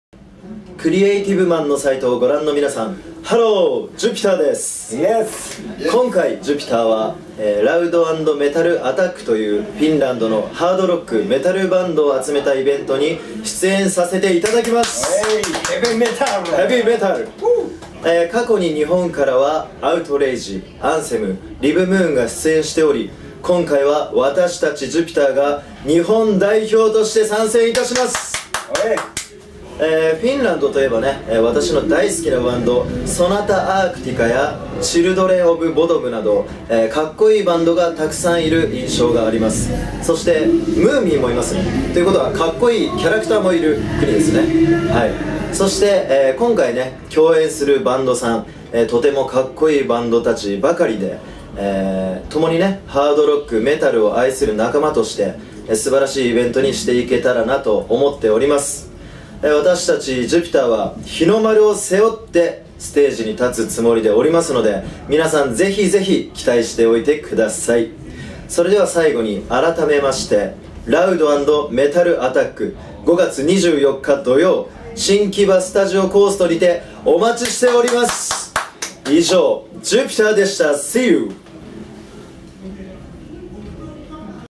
Jupiter video message to LOUD & METAL ATTACK
【LOUD & METAL ATTACK】
2014/5/24(土)東京 新木場STUDIO COAST

ACTS :
AMORPHIS / VON HERTZEN BROTHERS / STAM1NA / ARION
THE BLANKO / Jupiter(from Japan)